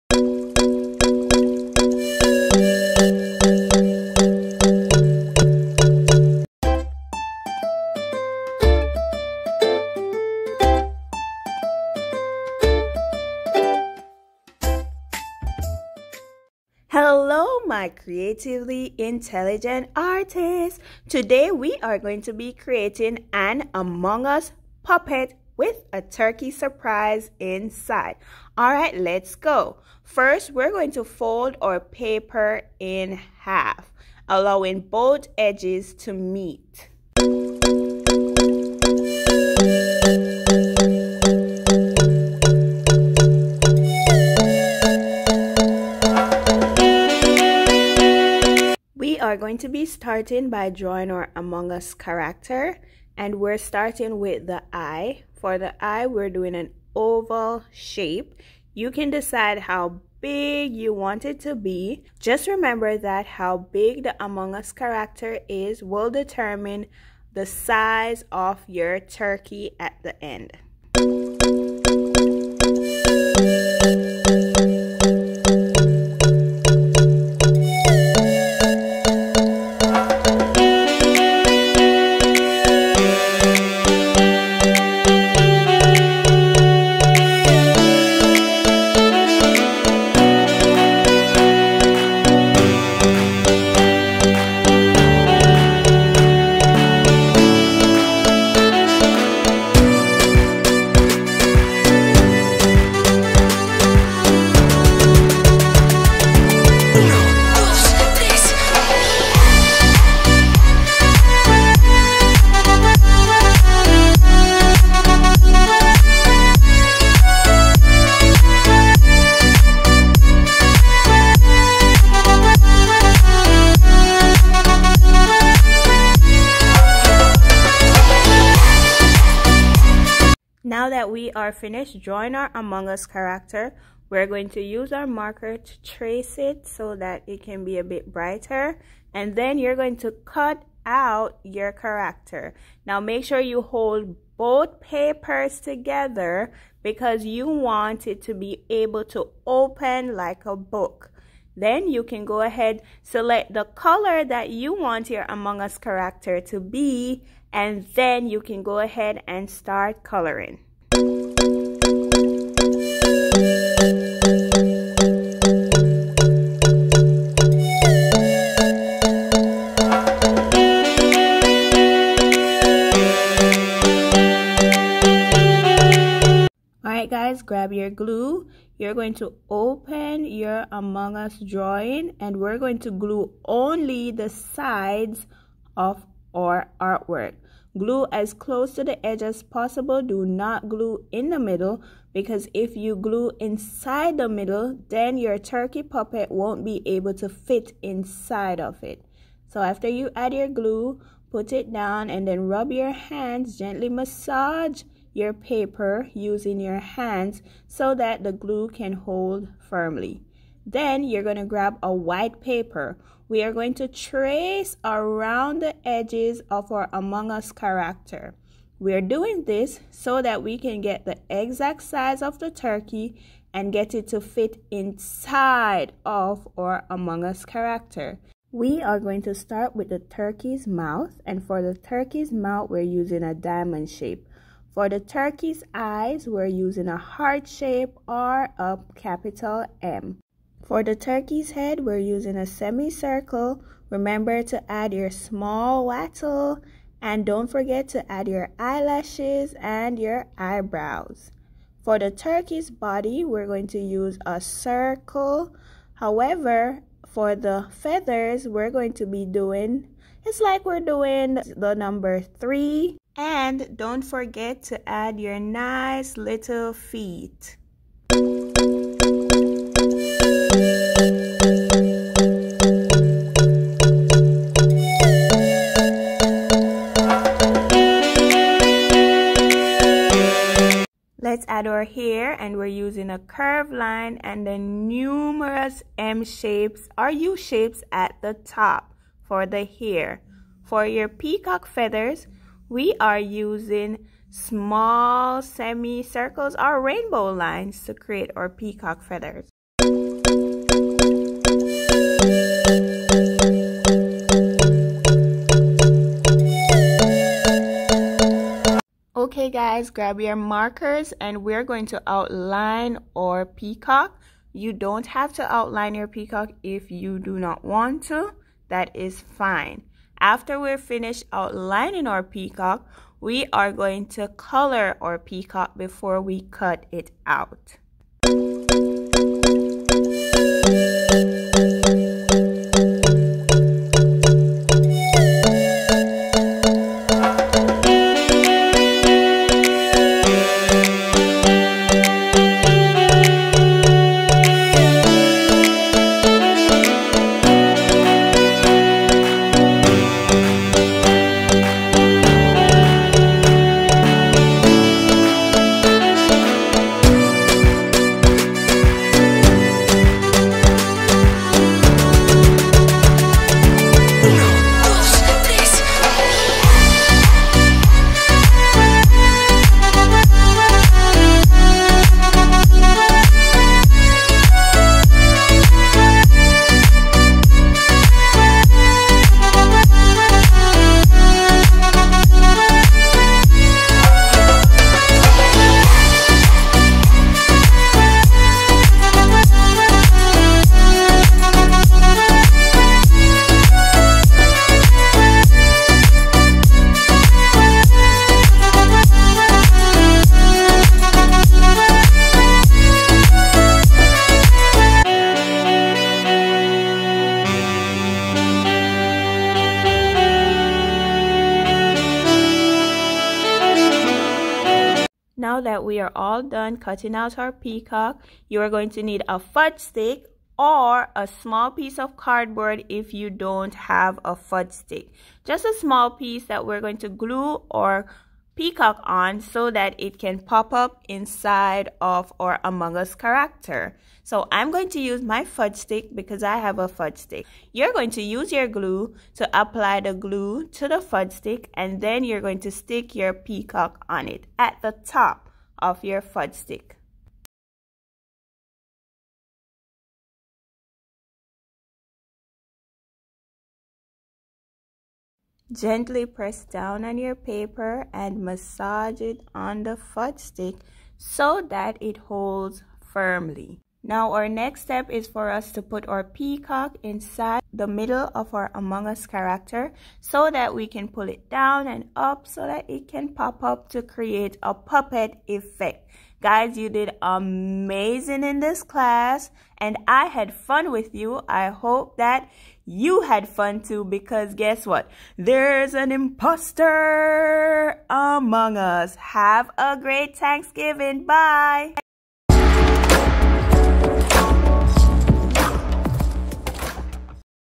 How to make an Among Us Puppet
Hello my creatively intelligent artists! There is an imposter among us! Let's make a cool Among Us Puppet and pops up into a turkey!